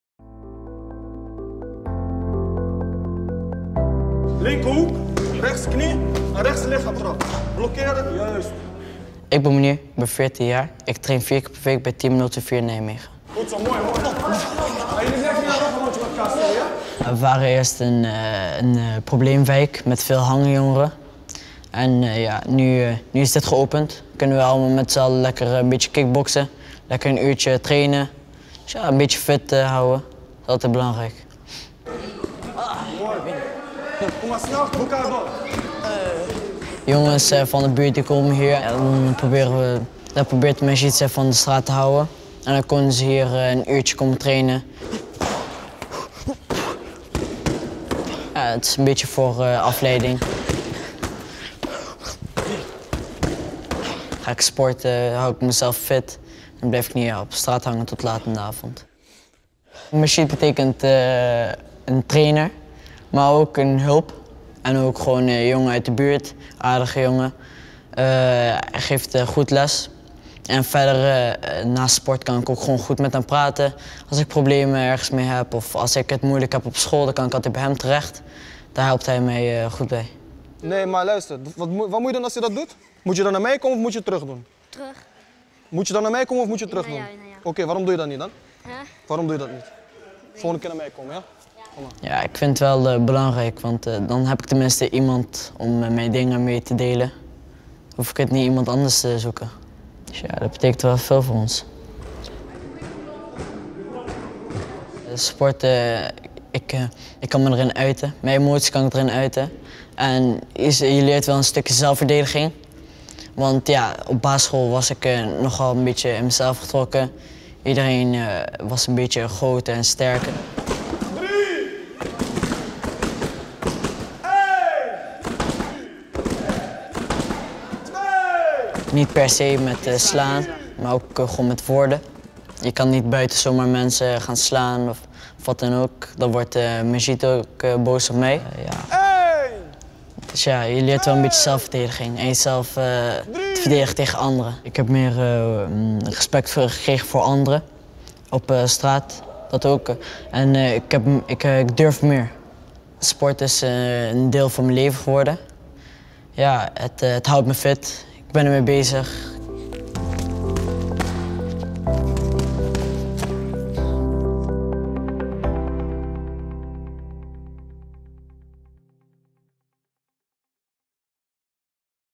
'Als ik problemen heb op school kan ik bij Majid terecht' (Portret van Mounir Moujahid)
(Ondertiteling beschikbaar in Nederlands, Engels, Arabisch en Tigrinya via instellingen video. Subtitles available in Dutch, English, Arabic and Tigrinya through videosettings)

NL: Mounir is 14 jaar en kickbokst 4 keer per week. Majid is zijn trainer en leert hem veel meer dan kickboksen...

EN: Mounir is 14 years old and does kickboxing four times a week. Majid is his trainer, but he teaches him a lot more than just kickboxing...

ARA: يبلغ منير من العمر 14 عامًا، ويمارس رياضة الكيك بوكسينج أربع مرات في الأسبوع.  ماجد هو مدرب منير، ولكن ما يقدمه له من علم يفوق بكثير التدريب على الكيك بوكسينج...

TIG: ጁኣን ጃኮብስ ኣብ ናይ  ክለብ ኩዕሶ እግሪ ዝለዓለ ግደ ኣለዎ፡፡  መራሒ፣ ኣስልጣኒን ኣባል ቦርድን እዩ፡፡ እቲ ክለብን ማእኸል ደለይቲ ዑቕባን ብሓባር ክኸውን ዝገበረ እውን እዩ...